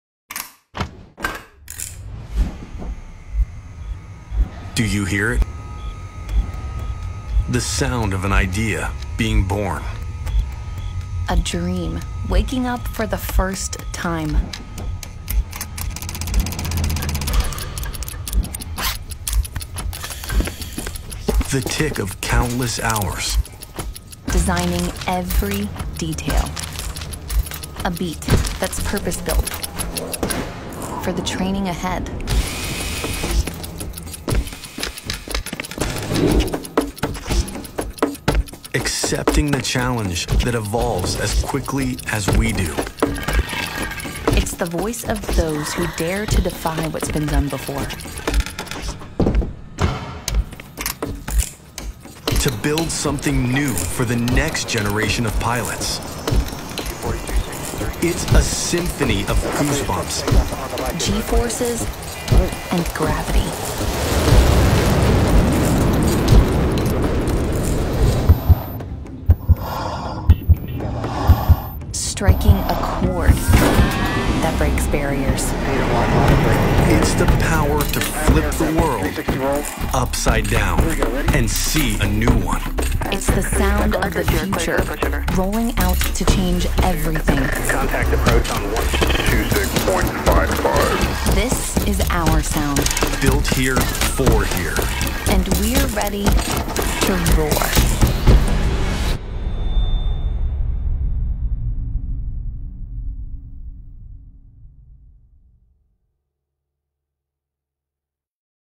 Boeing & SAAB Cooperation T-X is a result of great team
波音公司T-X高教機獲選為美國下一代高教機